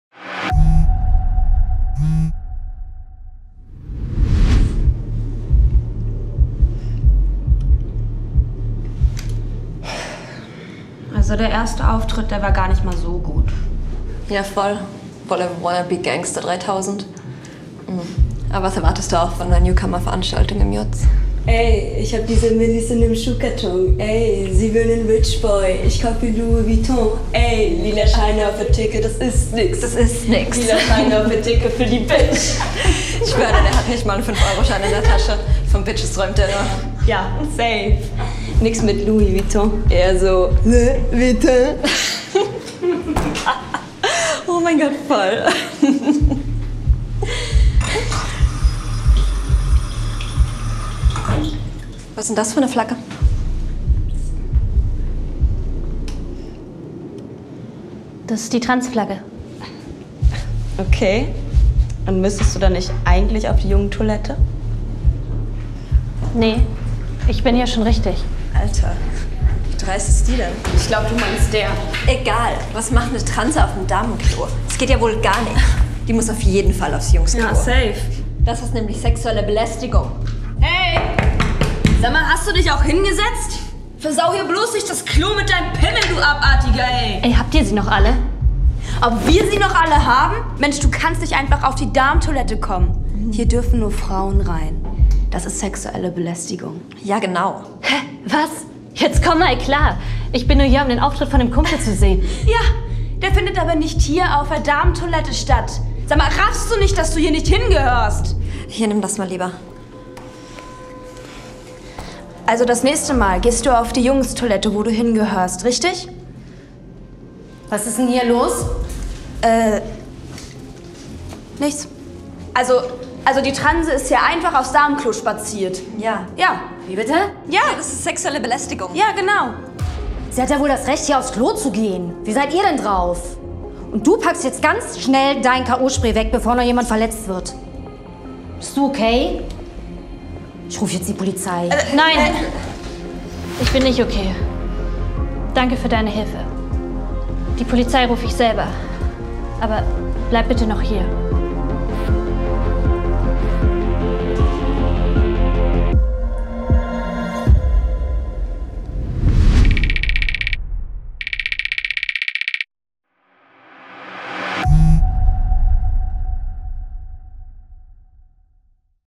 Luisa - übersetzt in Deutsche Gebärdensprache
CN: Sexuelle Gewalt und Transgender

Dieser Clip entstand im Rahmen des Projekts UNDDU? Mach Dich stark. Gegen sexuelle Gewalt unter Jugendlichen. Produziert von Herzschuss, in DGS übersetzt von Katja Fischer von FISCHSIGNS

Jugendliche der LGBTQI Community sind häufig von sexueller Gewalt betroffen. Wie können wir unterstützen? Und wie den Täter*innen Grenzen setzen?